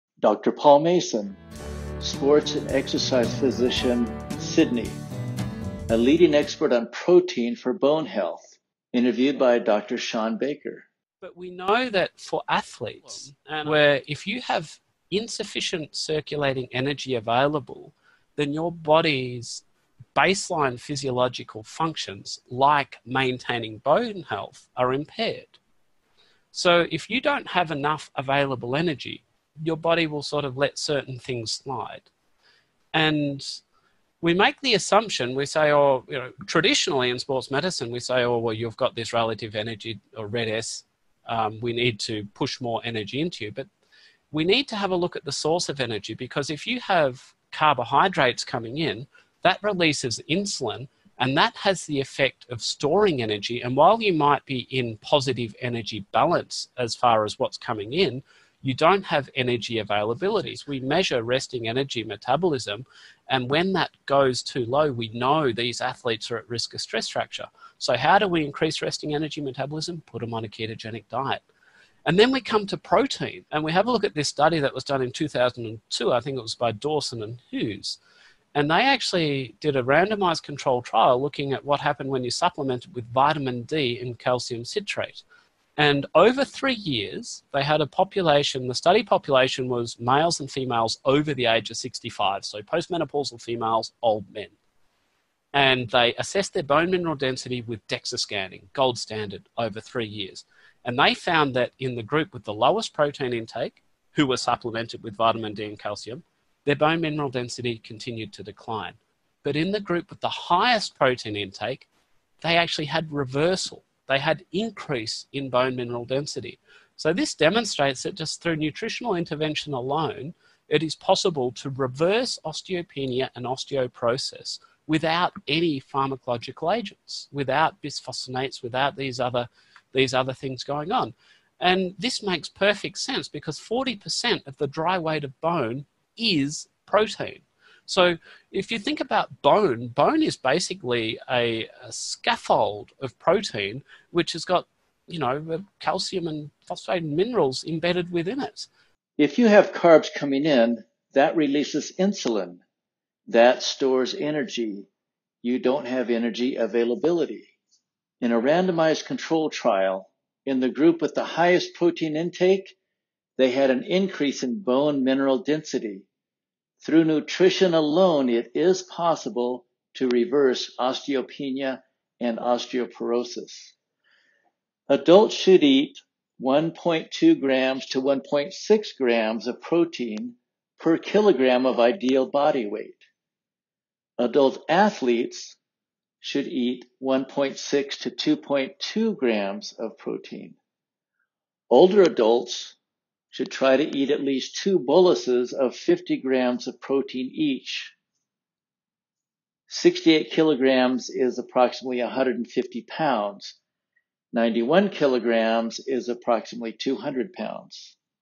Paul Mason: Protein & bone health
Dr Mason discusses protein's role in our bone health with Dr Shawn Baker. Nutrition, focusing on animal protein in the right amounts daily, can reverse osteopenia and osteoporosis.

This is a re-edit of the original HomeSchoolHero video.